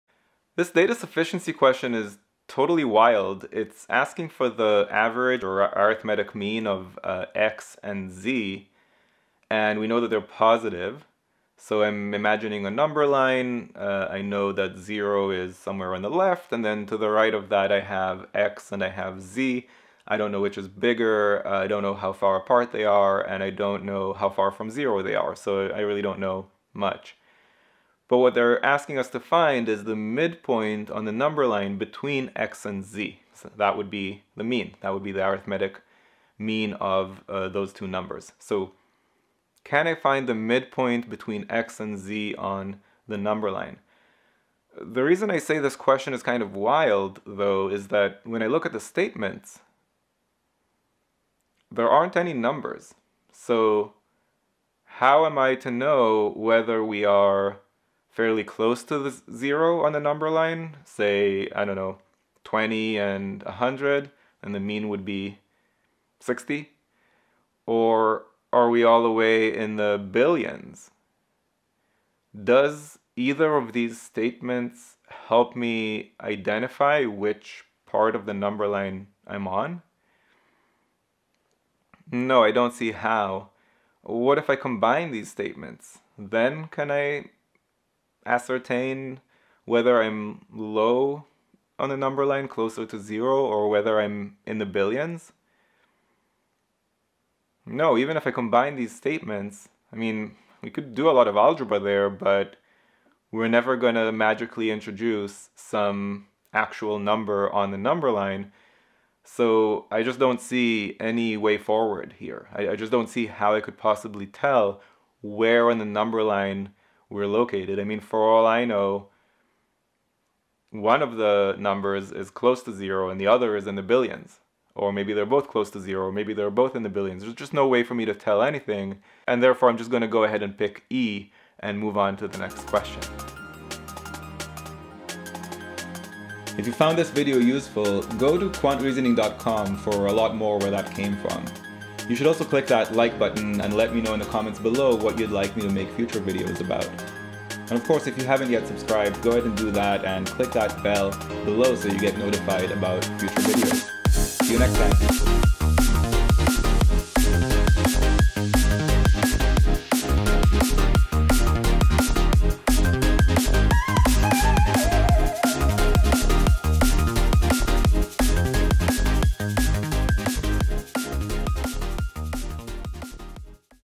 If x, y, and z are positive numbers, what is the value DS02706
GMAT DS02706

If x, y, and z are positive numbers, what is the value of the average (arithmetic mean) of x and z ?

(1) x − y = y − z
(2) x^2 − y^2 = z